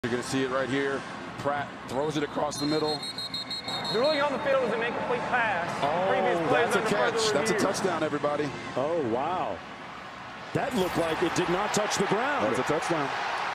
Tulane Wins Cotton Bowl late Touchdown over USC
Late Touchdown that was reversed after video review to give Tulane the lead with 9 seconds left